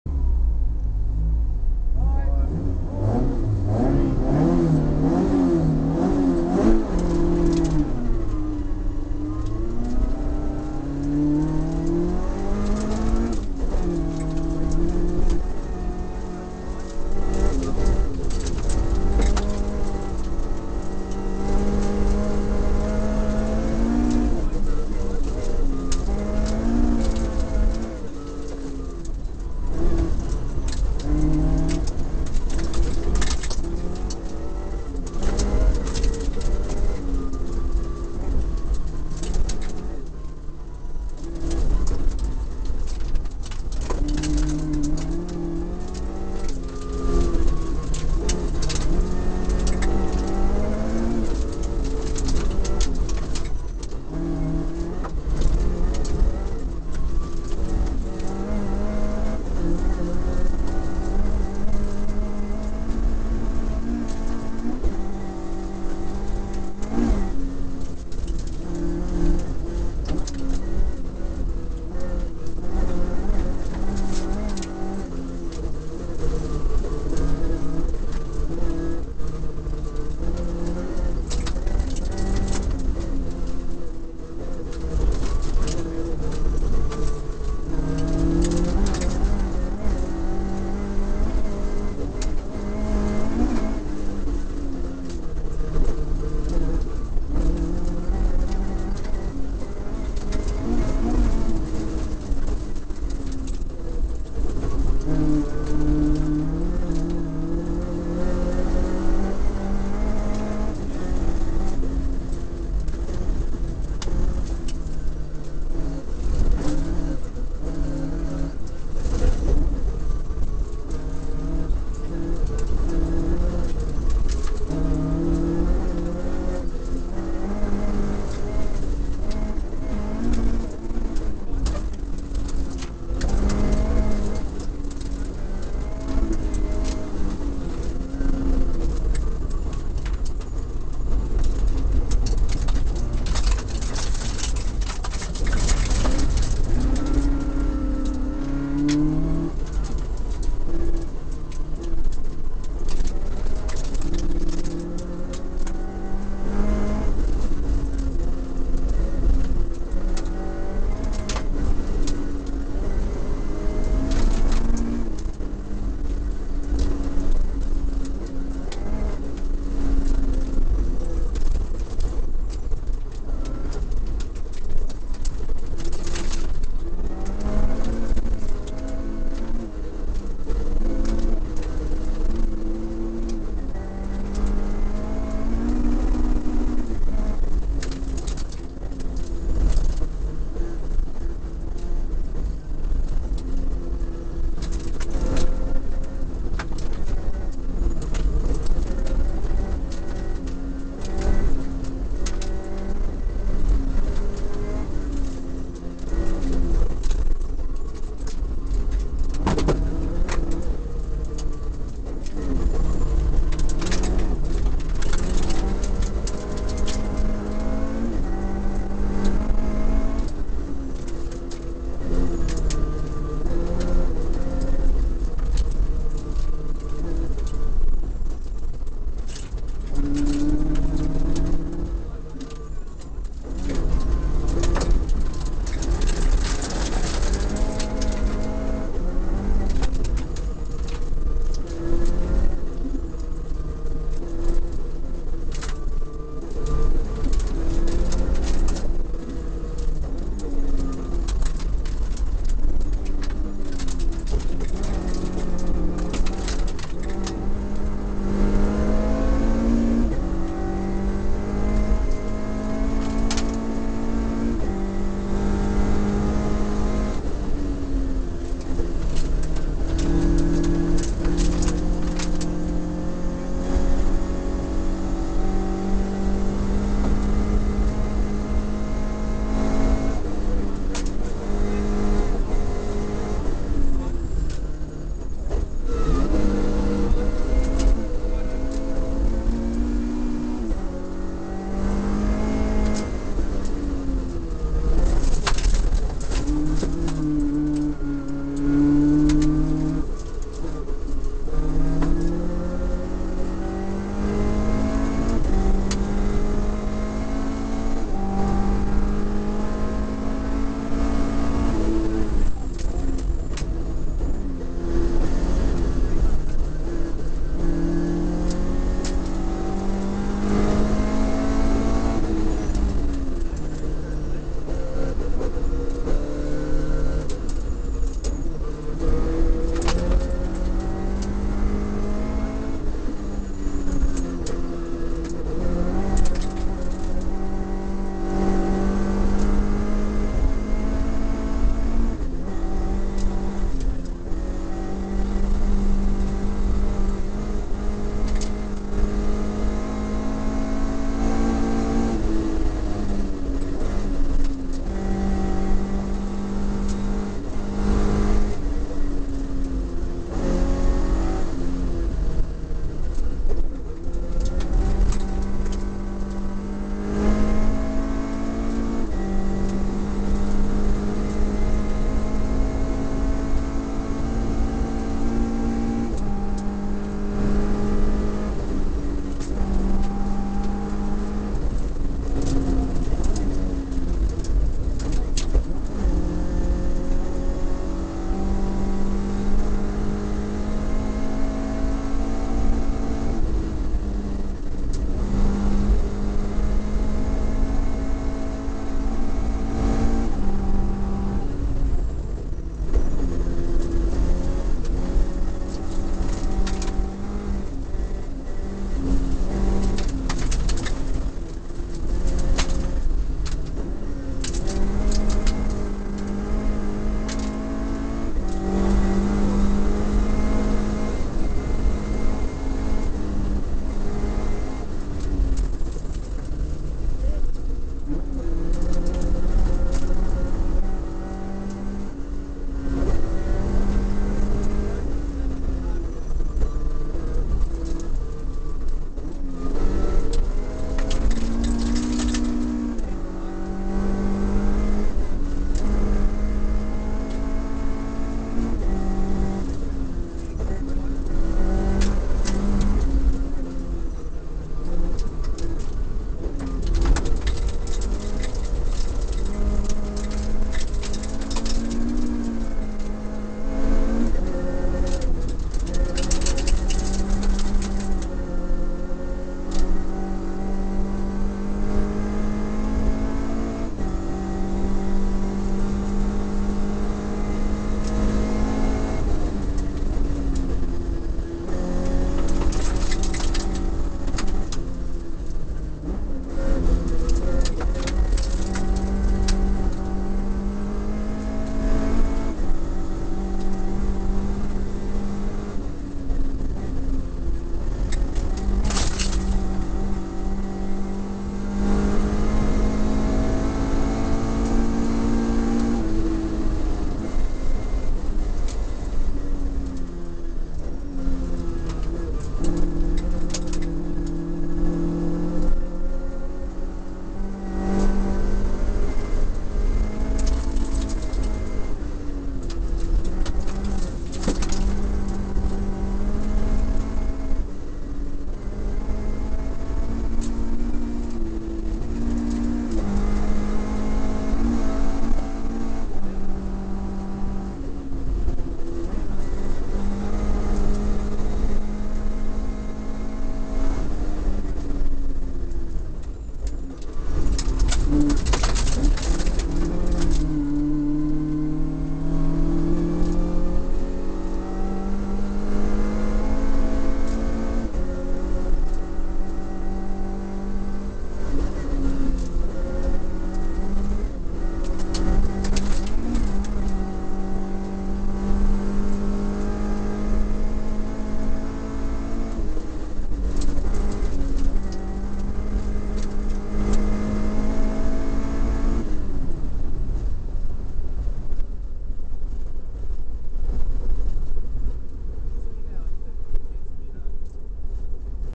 TRS 2010 Styx Valley Rally -SS2 Toyota Corolla Levin Coupe
Marcus Walkem & Tim Kulhanek on SS2 of the Styx Valley Rally